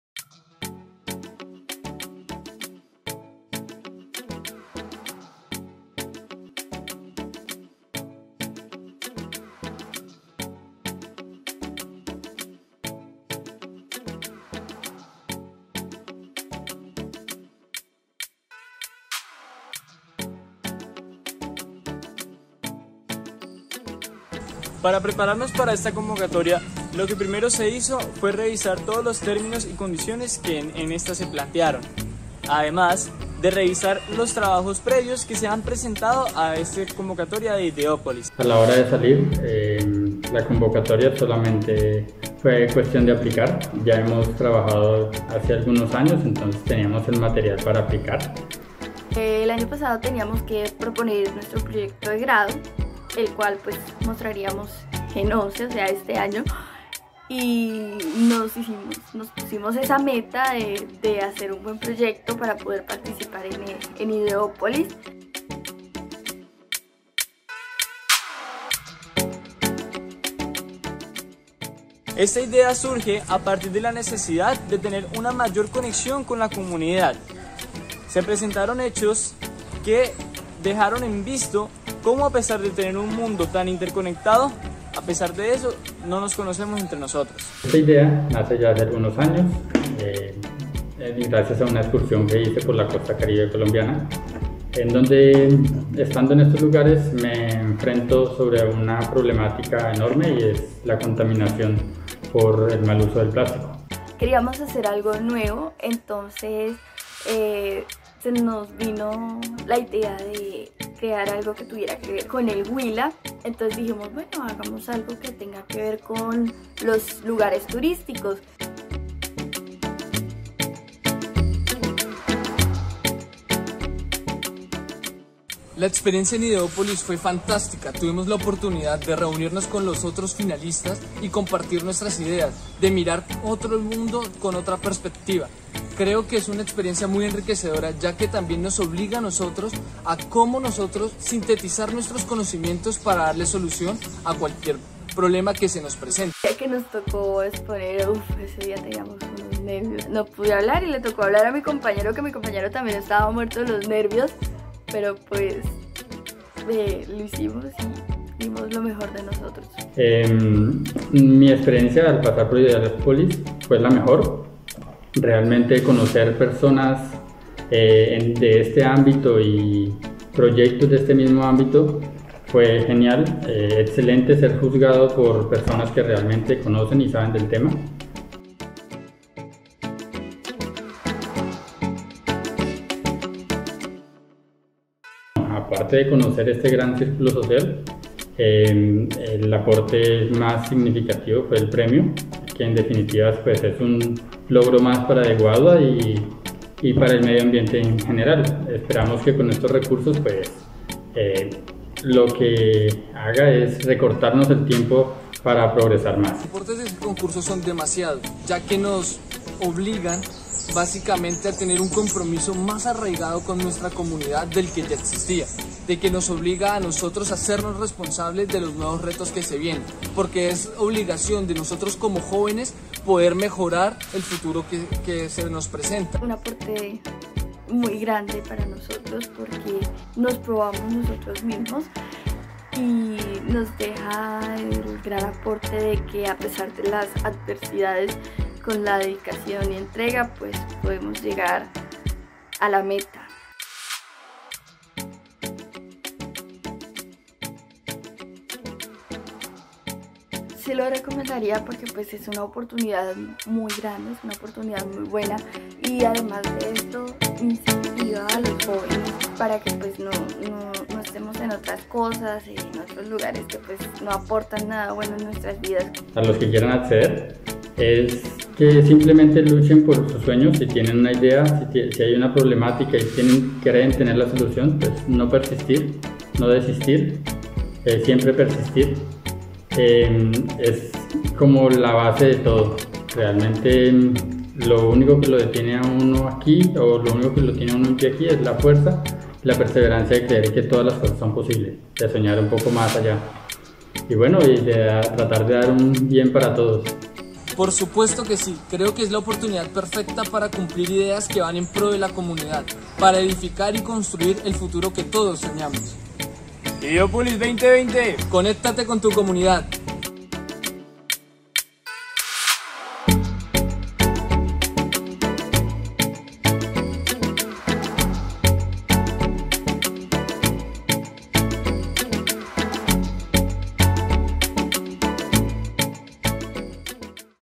Ideópolis 2020 - Capítulo Neiva
Conoce las experiencias de los ganadores de la versión 2020 del concurso Ideópolis Capítulo Neiva.